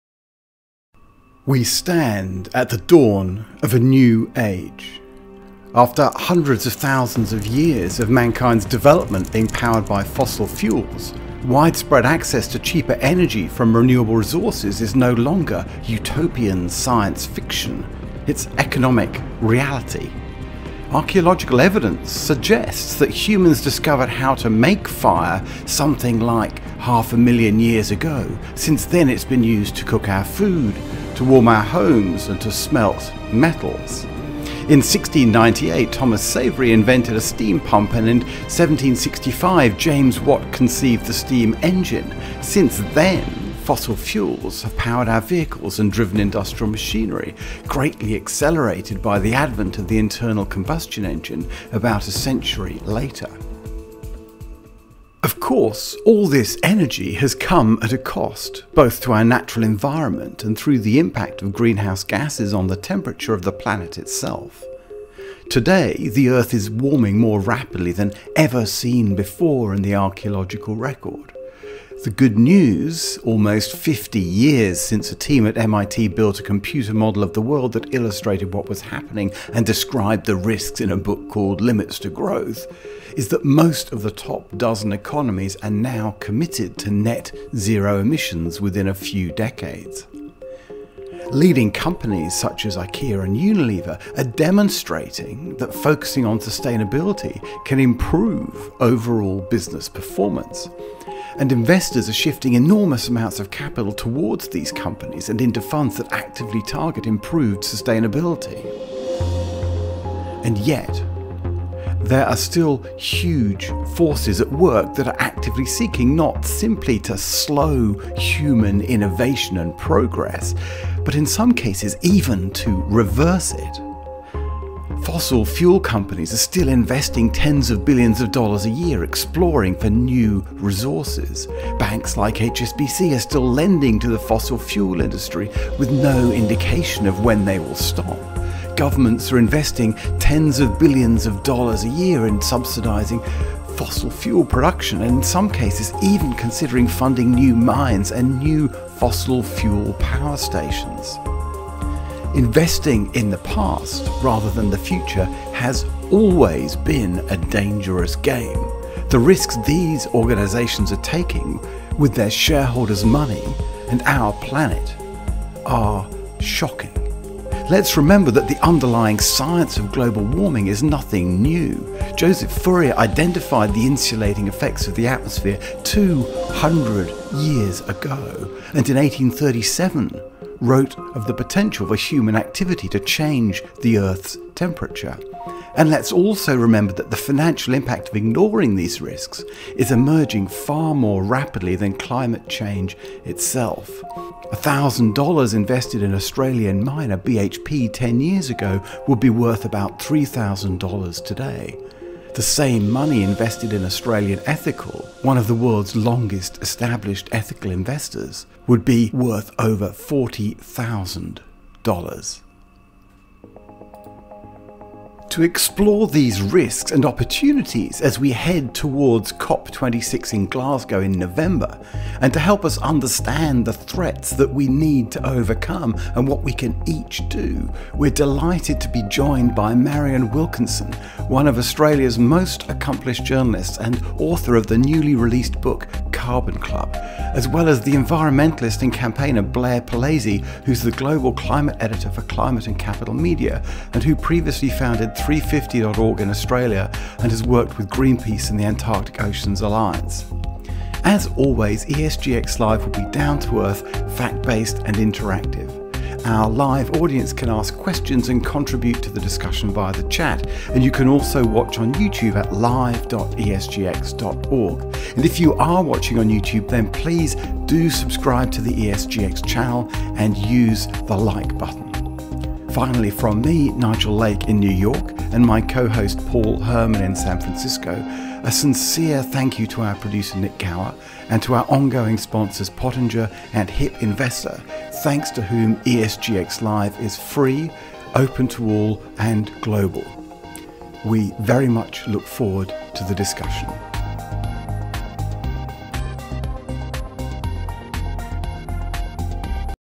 PREVIEW! ESGX 38 Carbon Club + COP 26 Preview with Marian Wilkinson and Blair Palese
Global attitudes to climate change are shifting. This isn't just a result of consumer pressure or political advocacy - the economics of power generation, transmission, distribution, storage and use are have changed dramatically too.

Globally, most of the top dozen economies have committed to net zero by 2050 (China by 2060), including Japan, the Eurozone, UK, South Korea and Canada.  Those that fail to act don't just look recalcitrant - they're also embracing significant commercial, financial and geopolitical risk.

Meanwhile, in the all-important US market, the Biden administration has re-entered the Paris Accord, and set policy to achieve a 100% clean energy economy and net-zero emissions no later than 2050.  Major motor manufacturers such as GM are on board, and already running high profile advertisements promoting the cause. And Tesla's share price continues to fly high.

In addition, as we approach the mid-point between the signing of the Paris Accord and 2030, as well as the 50 year anniversary of Limits to Growth, the UK is pursuing an ambitious agenda for the COP 26 meeting in November.  And yet, despite this encouraging backdrop, we also know that there are substantial corporate forces working hard to slow down action on climate change.

In this special episode of ESGX Live, we will discuss the following questions:
What should we expect from COP-26 in Glasgow?
Where are the biggest opportunities for impactful action?
Who stands in the way of change – and how should we respond?
We're delighted to be joined by special guests:
Marian Wilkinson, one of Australia's leading journalists and two-time winner of the country's Walkley Awards, to talk in part about her recently published book The Carbon Club
Blair Palese, an environmental activist, campaigner and journalist. Blair  founded 350.org in Australia and has worked in Australia, the UK and the USA in senior management roles organisations such as Greenpeace International and The Body Shop.
We anticipate announcing further guests shortly.
As always, this episode will be live and interactive, with a particular focus on practical actions that businesses, governments and individuals can take - and the commercial and other benefits of doing so.